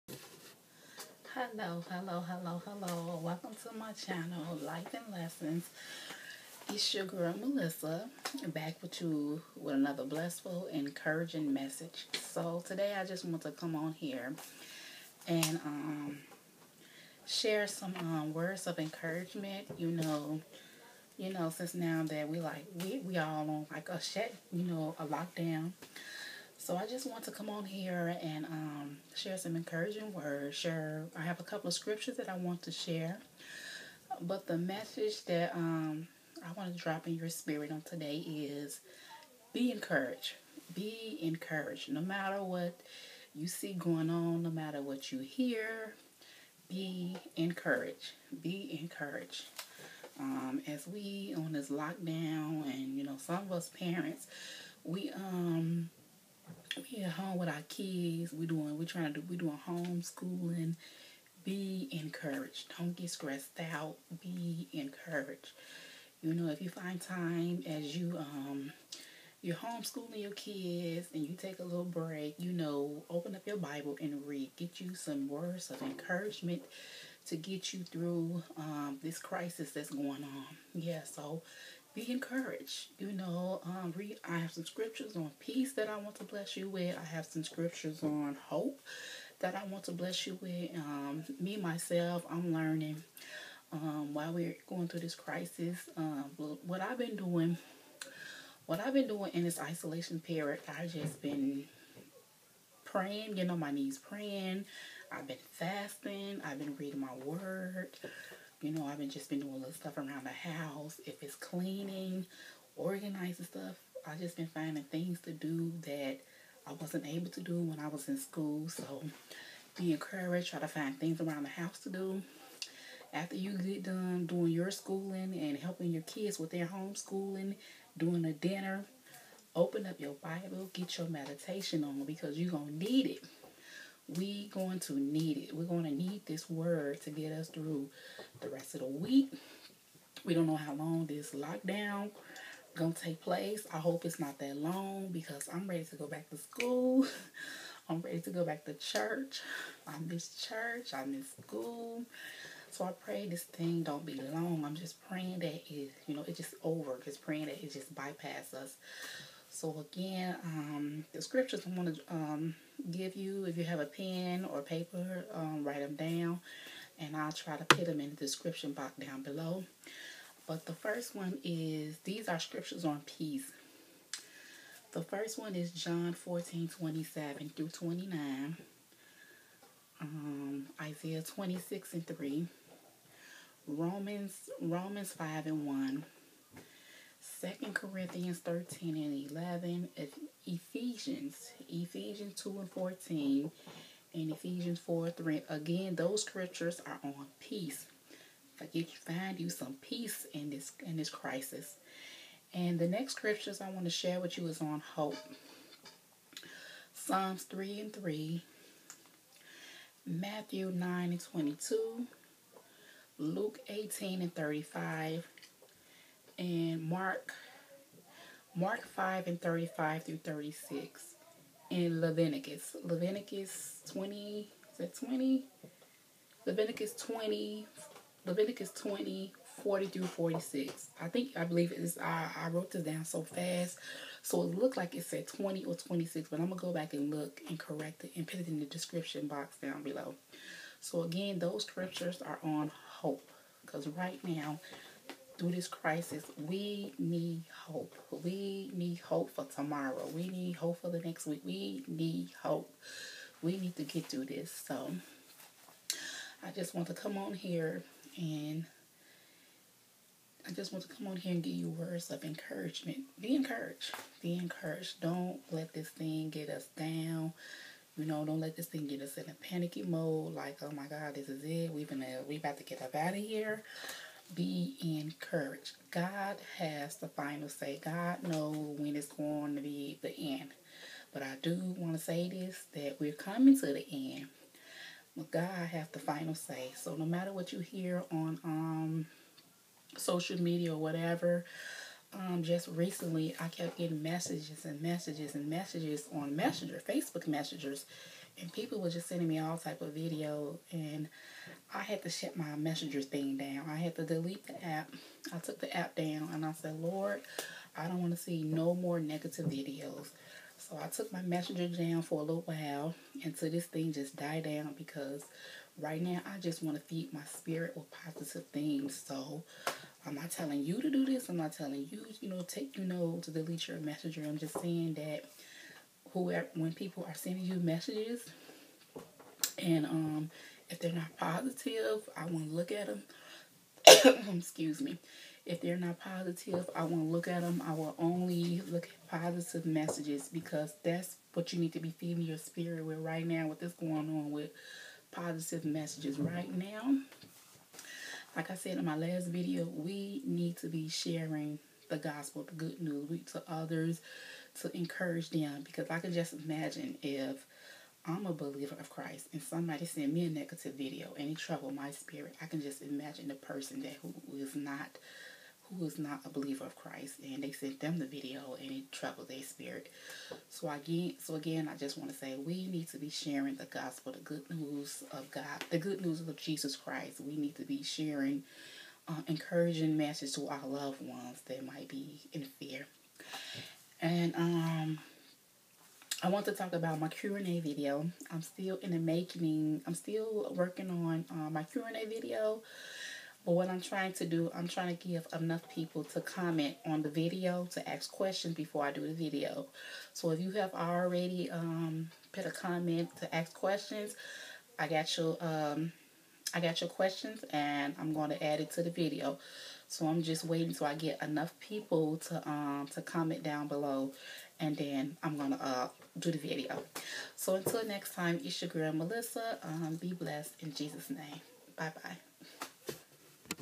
Life&Lessons| Be encourage😊🤗💞💞
Hello, Welcome to my channel Life& Lessons I just wanted to bless you guys with some words of encouragement to get you through this crisis that we are facing right now here are some scriptures.

(Peace)
John 14:27-29
Isaiah 26:3
Romans 5:1
2 Corinthians 13:11
Ephesians 2:14
Ephesians 4:3

(Hope)
Psalms 3:3
Matthew 9:22
Luke 18:35
Mark 5:35-36
Leviticus 26:40-45     Be blessed in Jesus name 😊